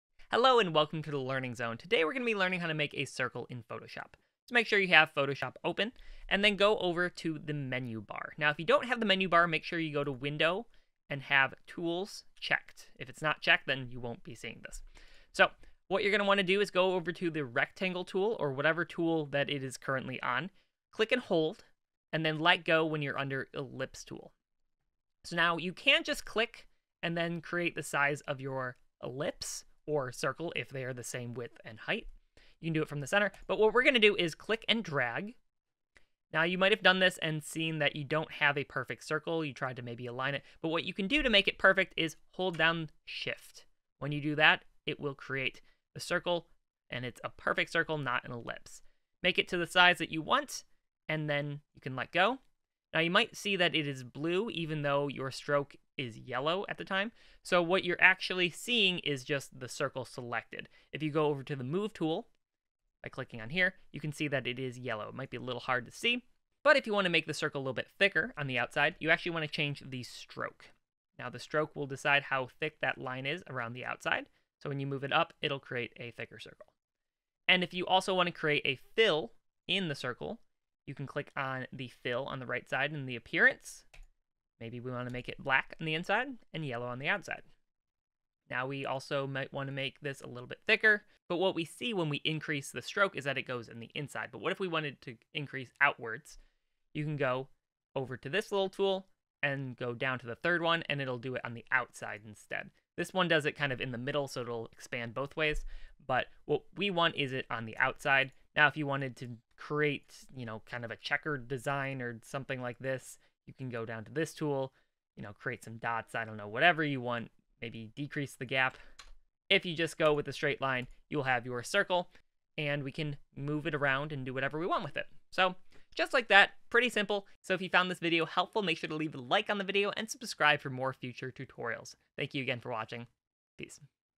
Create a Circle in Adobe Photoshop - 2022 Step by Step Tutorial
Today we are learning how to create a circle in Adobe Photoshop 2022.  This step by step tutorial should show walk you through without any problems.